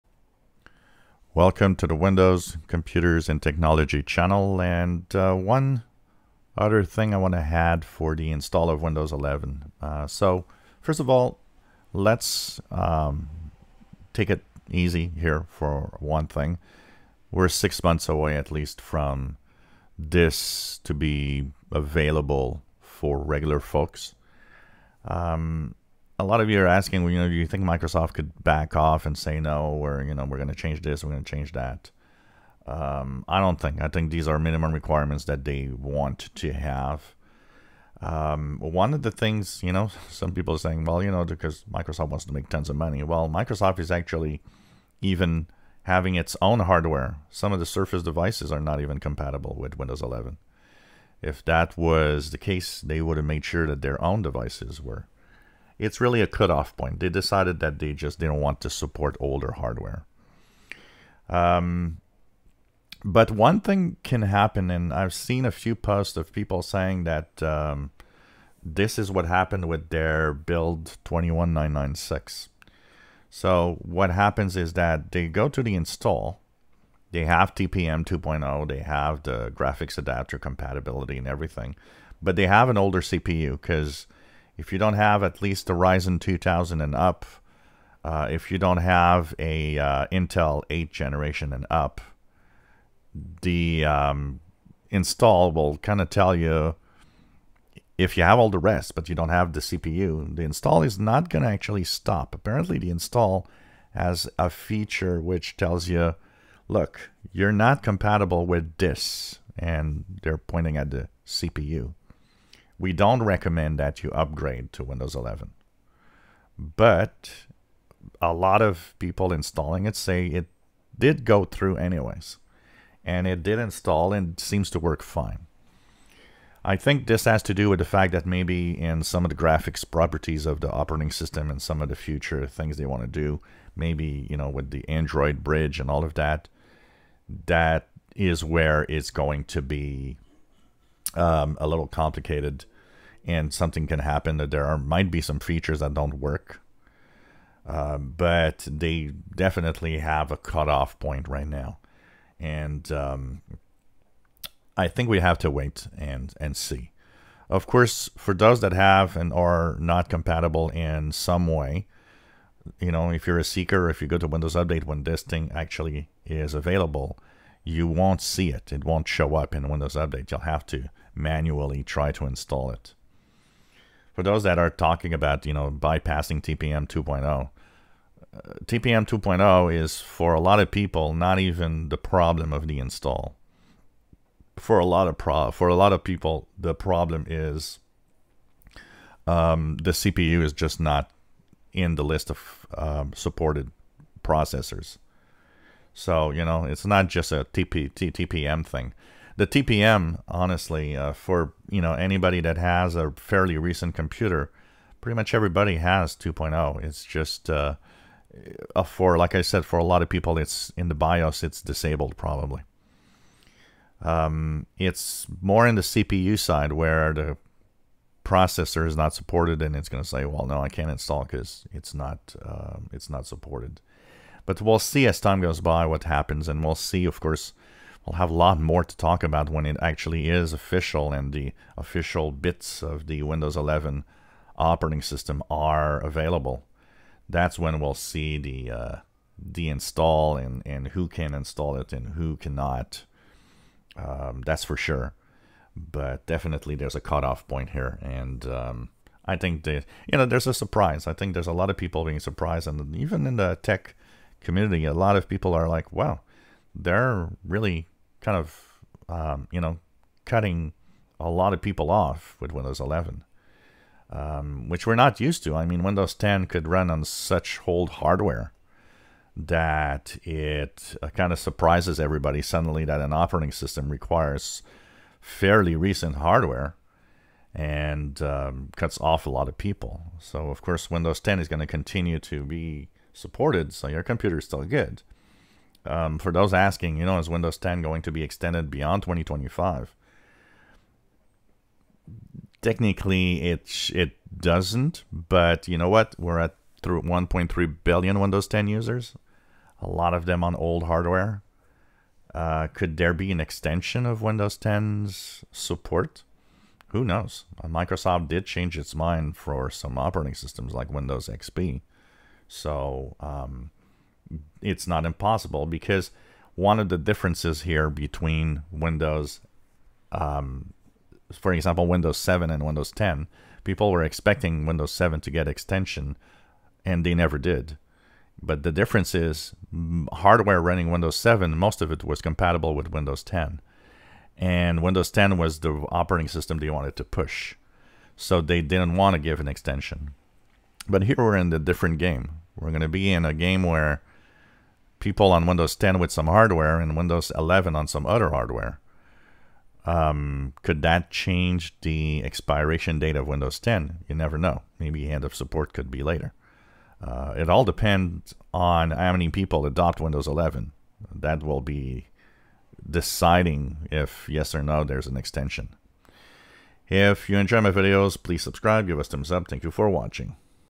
Windows 10 Windows 11 Compatibility installs and personal comments
More than TPM 2.0, CPU Compatibility might be the biggest problem.  Windows 10 will run older hardware